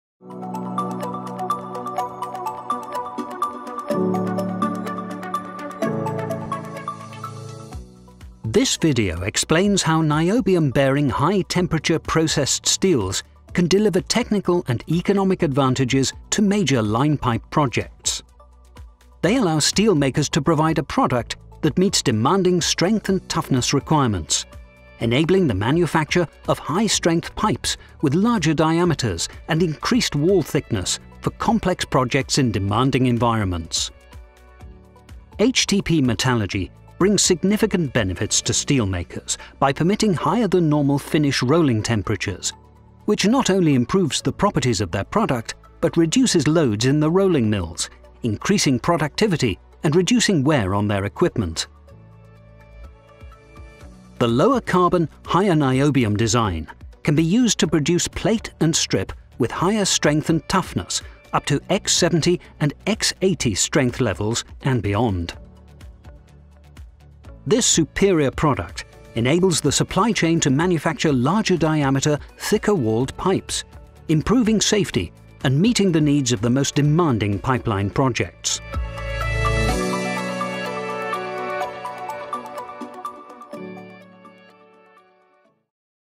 Niobium Technology - Meeting the Challenges of Global Energy Supply (CLIP) (2019)
Learn more about the HTP concept and how it has been applied in linepipe steels.